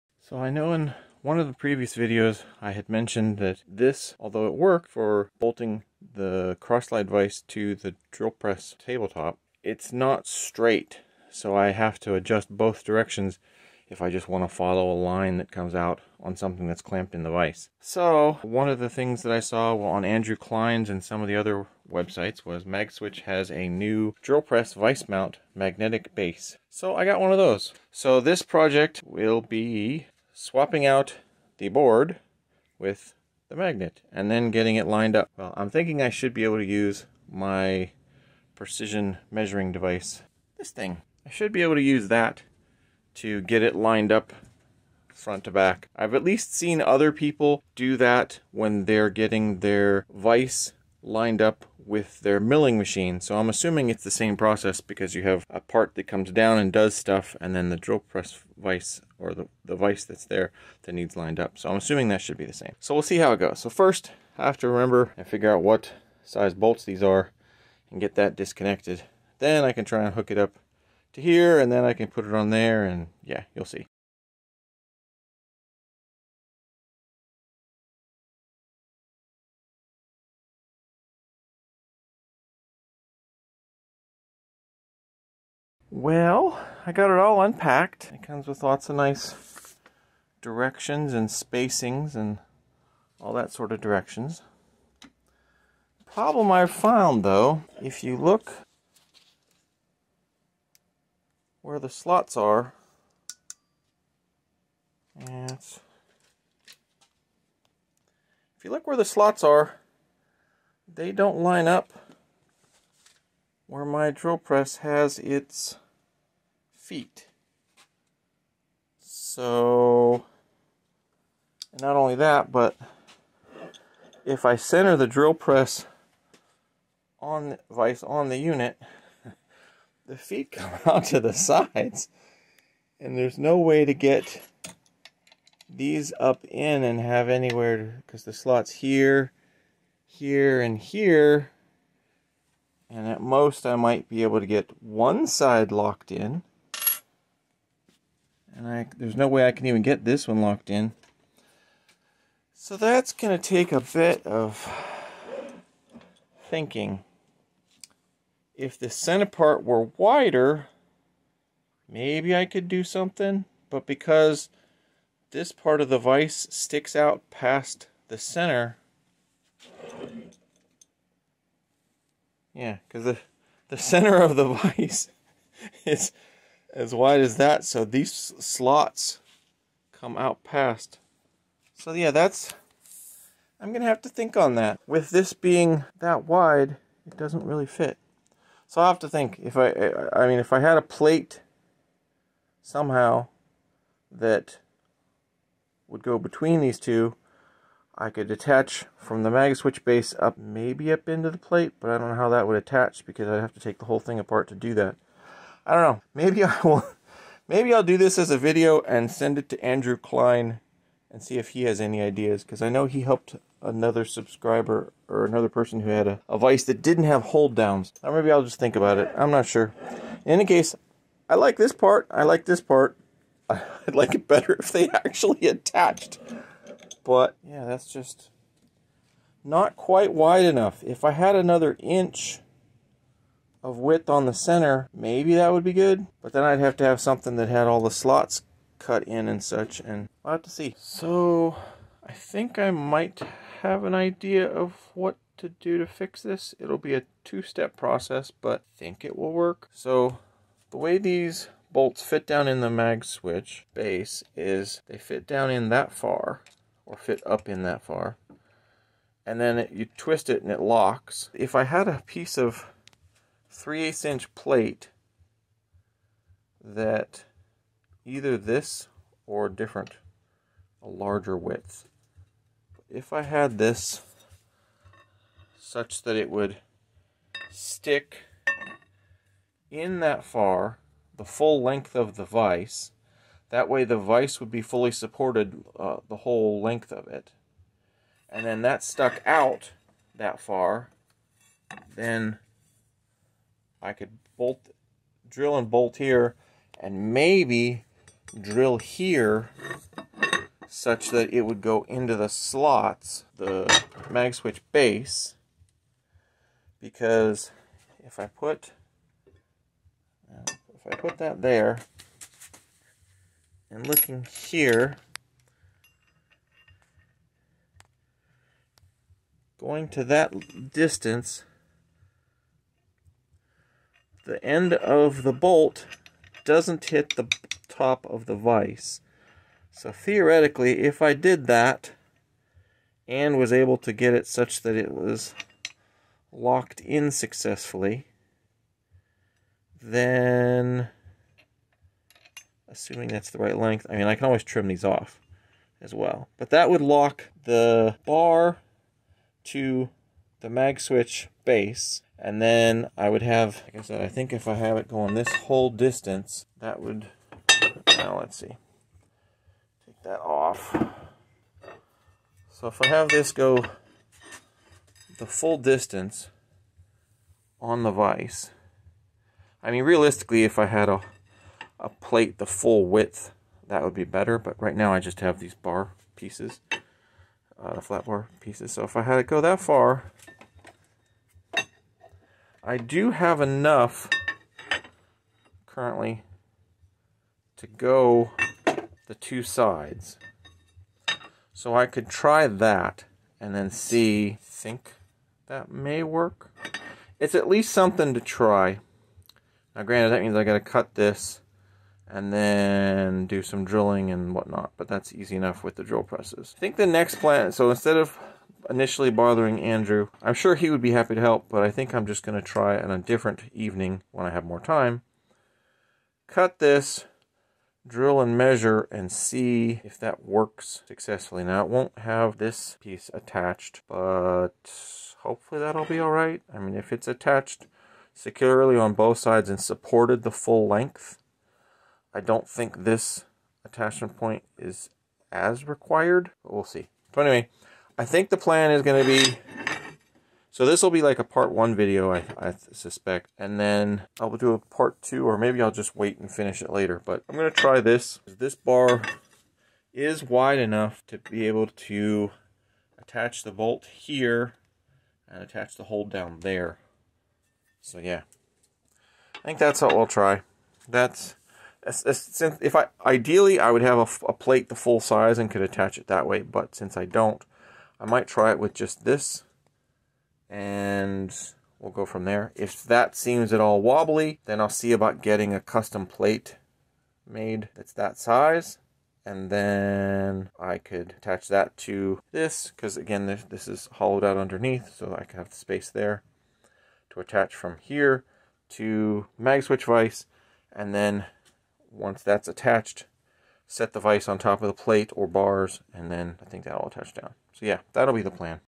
Magswitch drill press vice mount part 1
Since my current setup for my 6" cross slide drill press vice is not ideal, especially not easily adjustable, I decided to buy the new Magswitch drill press vice mount using the discount code from Andrew Klein's video:

The one thing I realized though, is my vice is too wide to fit the base without an intermediate plate.  So this is the first part showing the problem and the idea to solve it...I will have to make another video for the actual connection solution as I ran out of time this weekend to cut up and drill the metal bar.